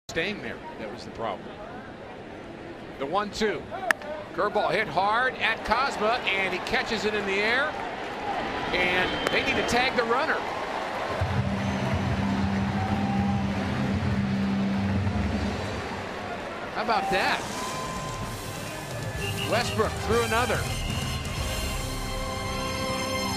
2013/04/27 Cards turn two twice
PIT@STL: Cards turn an unusual double play in fifth